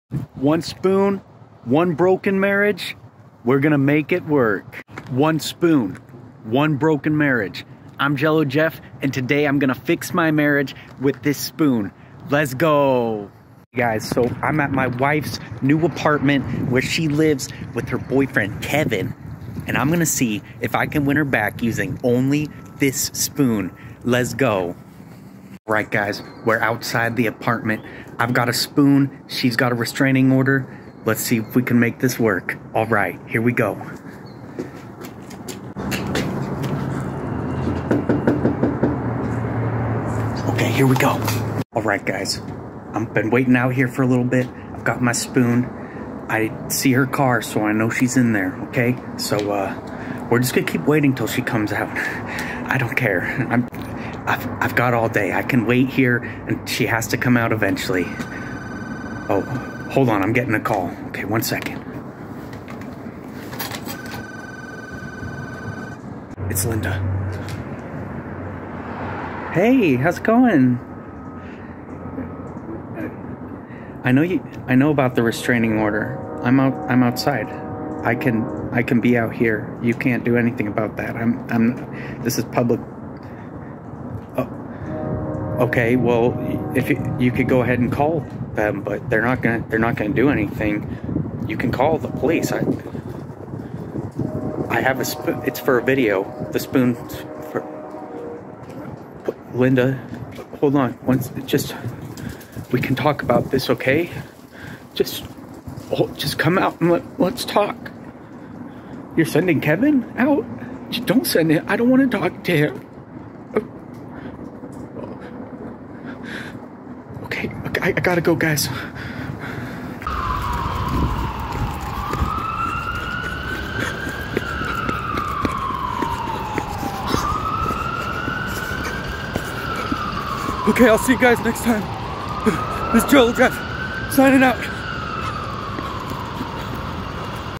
saving my marriage using only a spoon (POLICE CALLED)
Can I save my broken marriage using only a spoon, or will I end up arrested for violating a restraining order? FIND OUT!